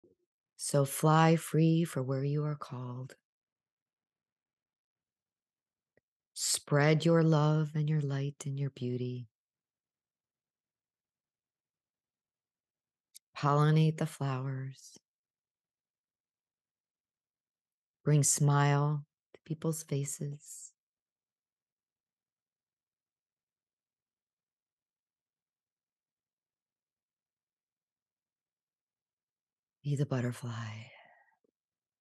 Butterfly heart is a transformative emotional state of being that interacts with the environment.
The meditation on the Butterfly Maiden is about the opening of the heart chakra's energy field.  There is a feeling that can be described as the butterfly heart which has a feeling of buoyancy.  You feel as if you are able to float across the ground.  The butterfly wings are the extension of the energy field from the heart center outwards that feels like etheric wings.

When listening to the meditation, it is best to have the intention to attune your frequency with that of the bliss charged frequency of your heart that produces the extension of the field and the feeling of wings.  The innate body consciousness knows this frequency and understands the frequency that you are wishing to produce and the affect that you wish to experience.

This is an excerpt of the meditation in the link at the end of the video